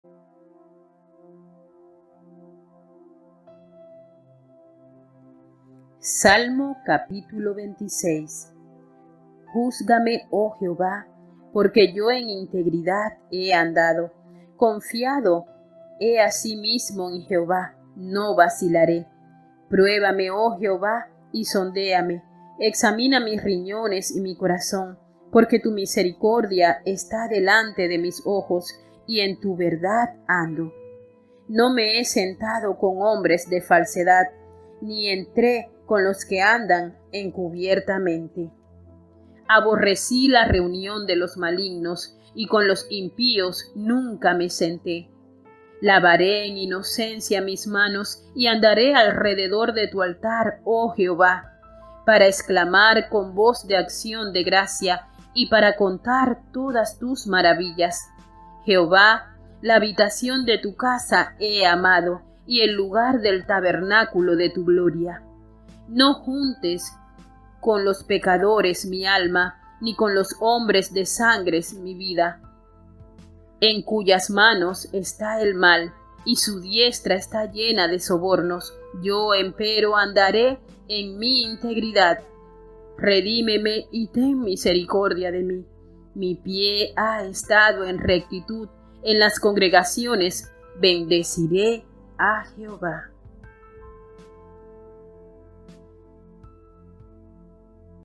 Biblia: Salmos 26 - Reina Valera Antigua - Mirla Royero
Voz: Mirla Royero Tovar
De: Colombia
Colaboradores: